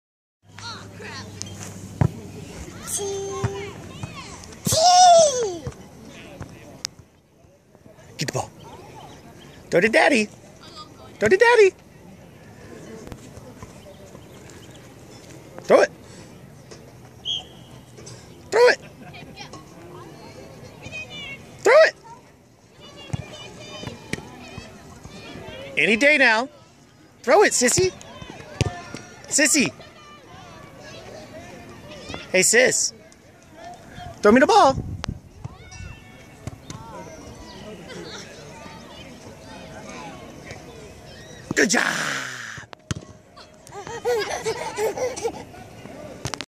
Addy Tosses Tennis Ball
Addy tosses a tennis ball...eventually.